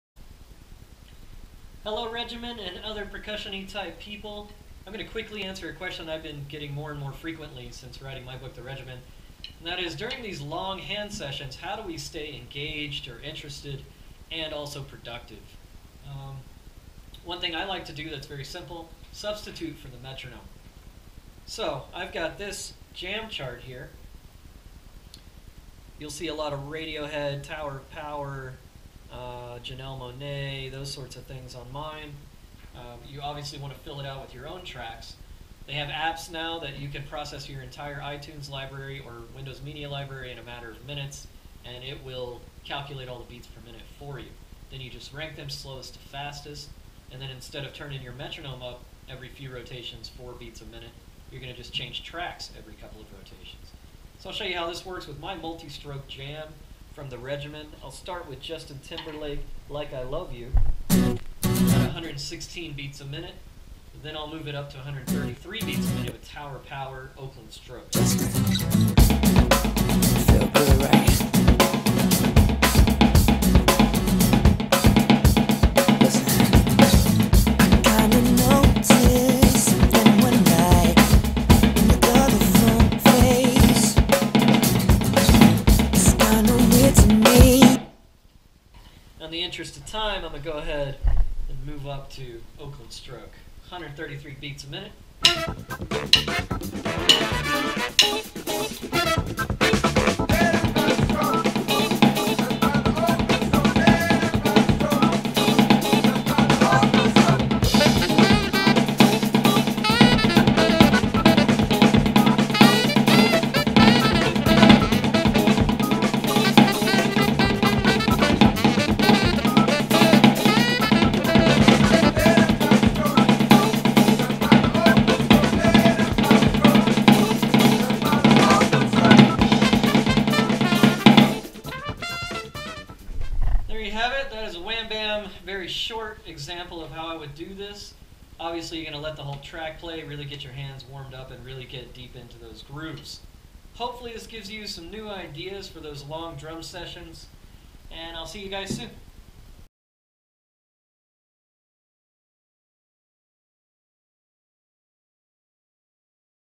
Multi Stroke Jam with Back Track
Using some tricks to keep yourself interested and improve your groove! Featuring Justin Timberlake and Tower Of Power.